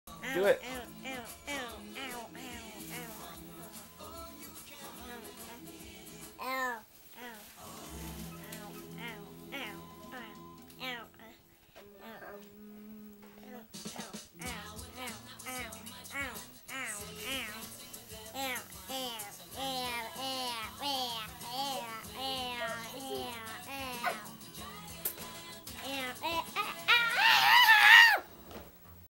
Joy Tortures Her Frog
Joy got a stretchy frog toy in her Easter basket. Poor frog.